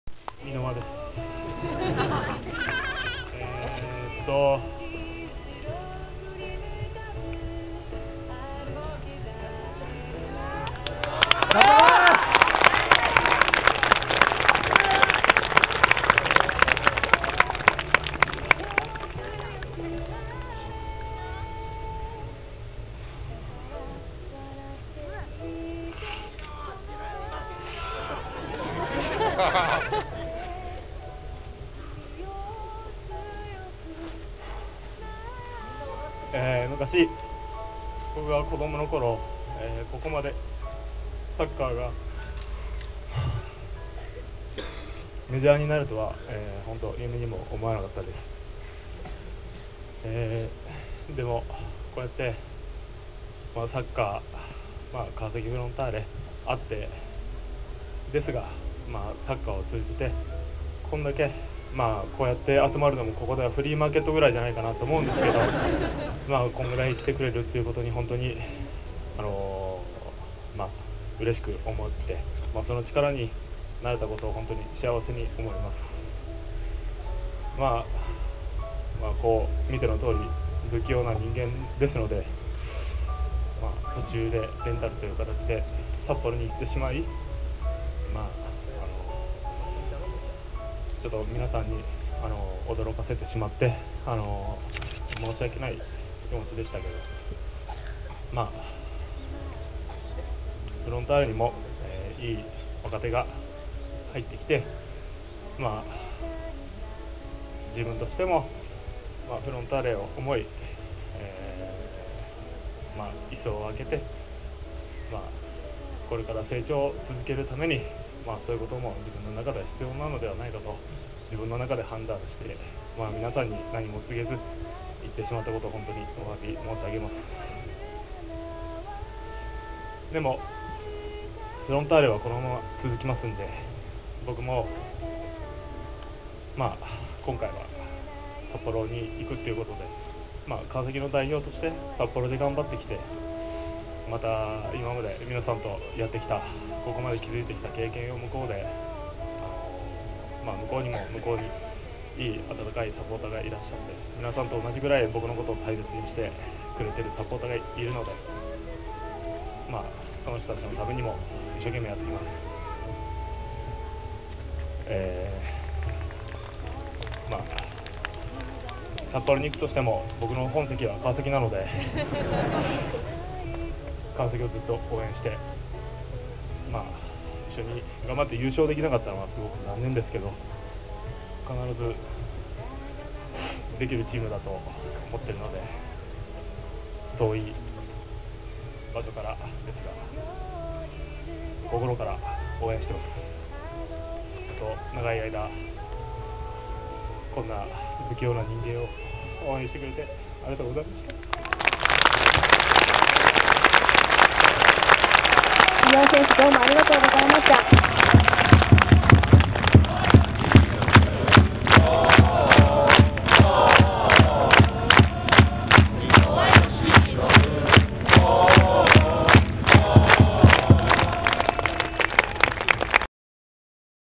川崎フロンターレ 契約満了選手送別会 #3 箕輪選手後半
2008/12/13 等々力緑地内で行われた選手送別会の様子
箕輪義信選手からサポーターへのご挨拶
コンサドーレ札幌へ移籍した心境等